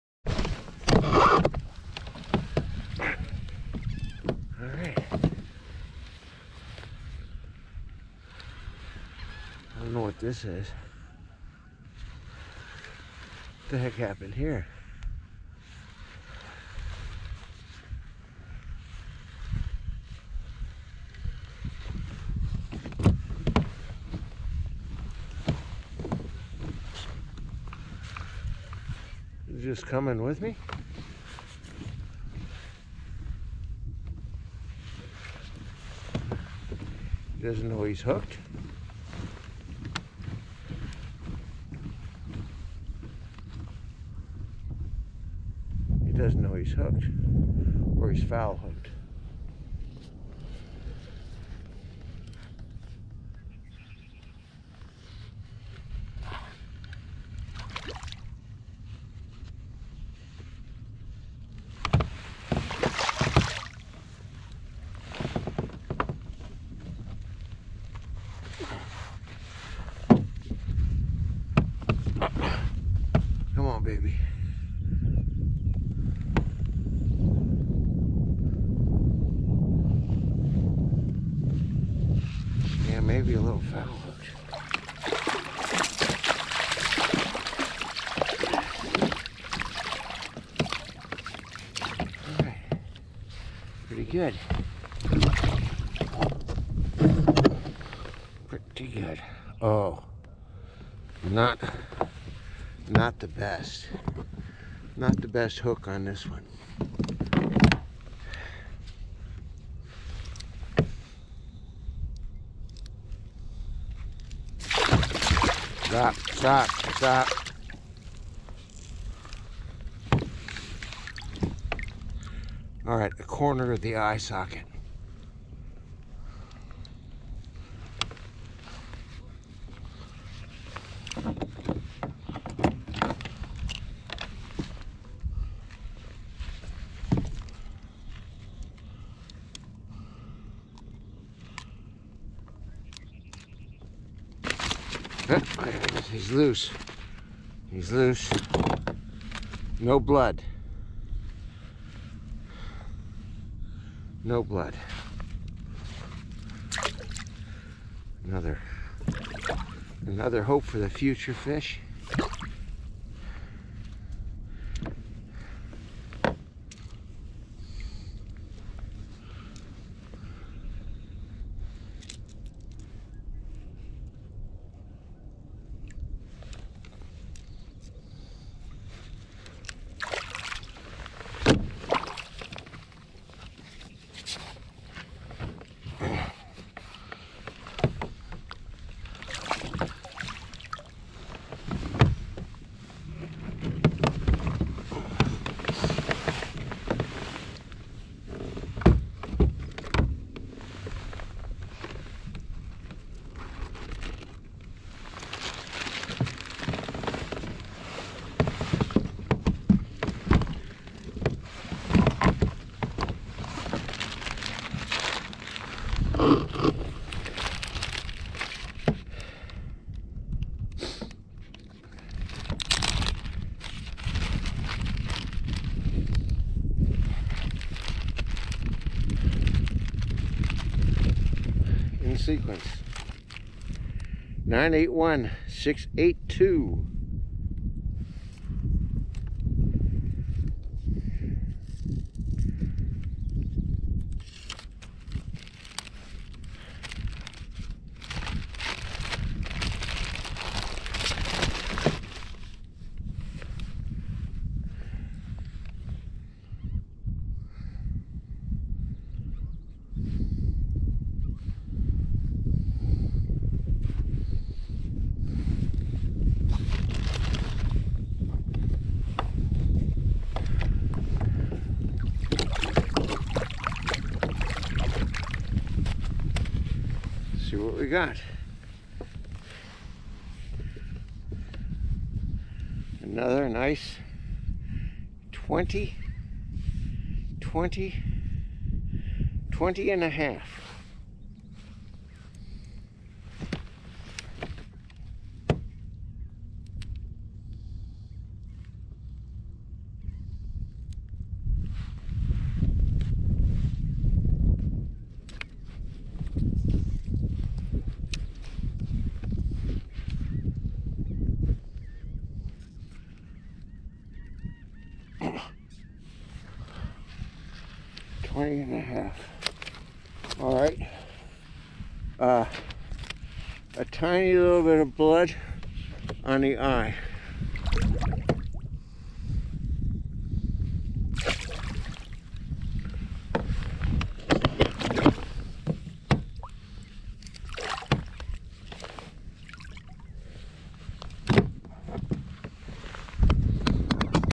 Tagging fish GH016676
Using video to capture data for Massachusettes Mortality survey